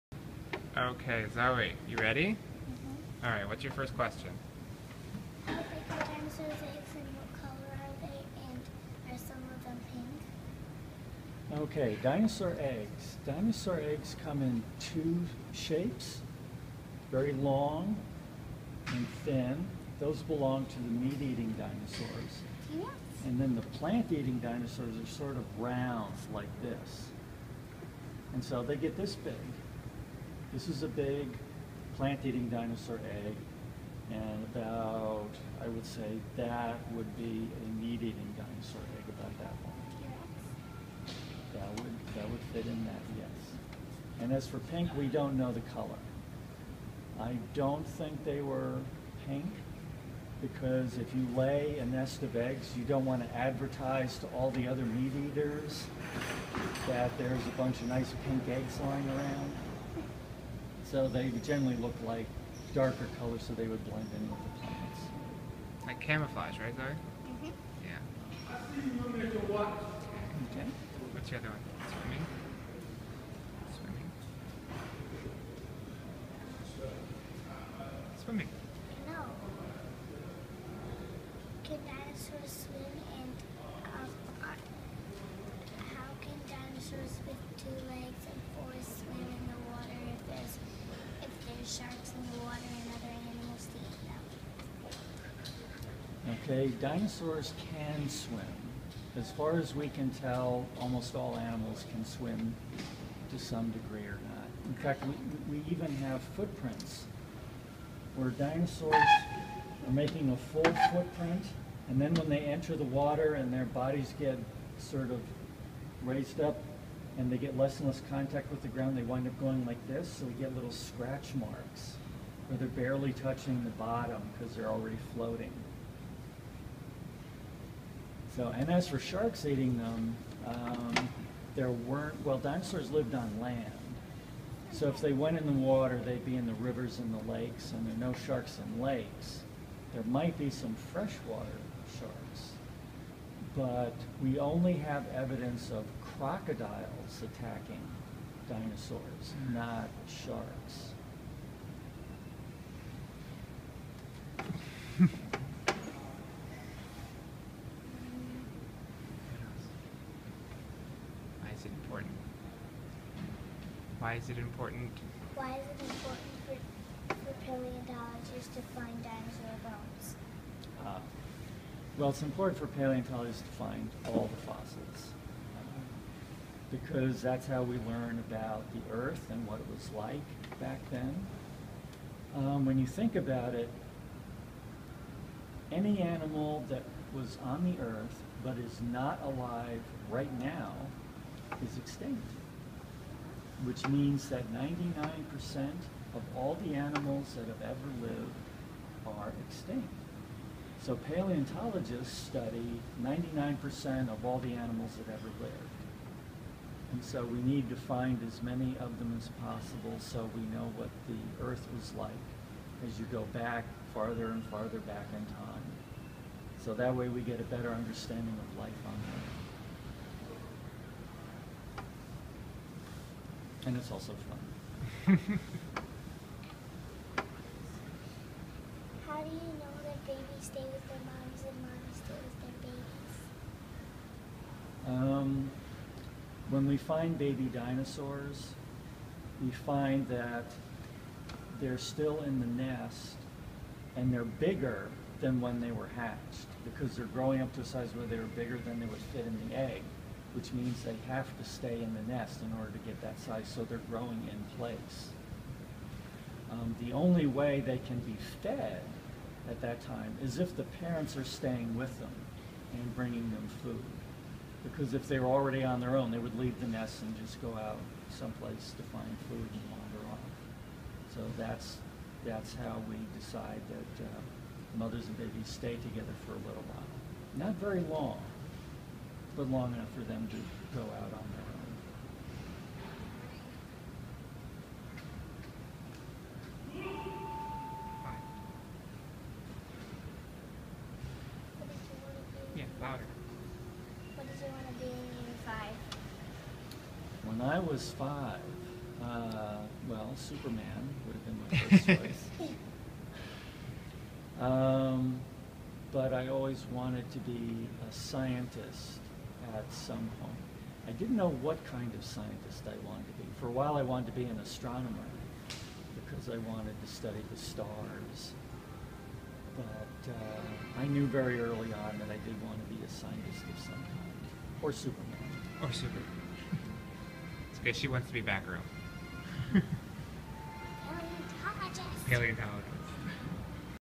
Zoey interviews a Smithsonian paleontologist
Zoey interviews Dr. Michael Brett-Surman, a paleontologist at Smithsonian's National Museum of Natural History.